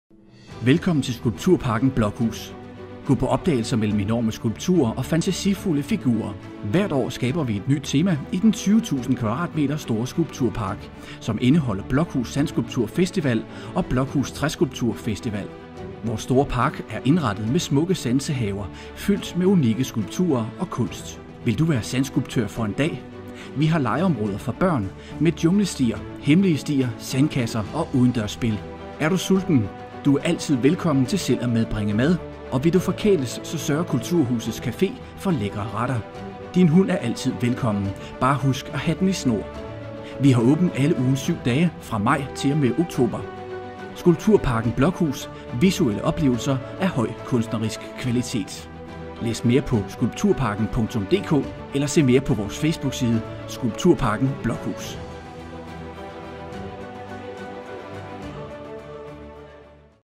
Skulpturparken Blokhus - Blokhus sandskulpturfestival & Blokhus Træskulpturfestival
Hvert år skaber vi et nyt tema i den 20.000 m2 store skulpturpark som indeholder Blokhus Sandskulptur Festival og Blokhus Træskulptur Festival.
Vores store park er indrettet med smukke sansehaver fyldt med unikke skulpturer og kunst.
Vi har legeområder for børn - med junglestier, hemmelige stier, sandkasser og udendørs spil.
Er du sulten? Du er altid velkommen til selv at medbringe mad og vil du forkæles, sørger Kulturhuset Blokhus - caféen for lækre retter.
Din hund er altid velkommen, bare husk at have den i snor.
Vi har åben alle ugens 7 dage fra maj til og med oktober.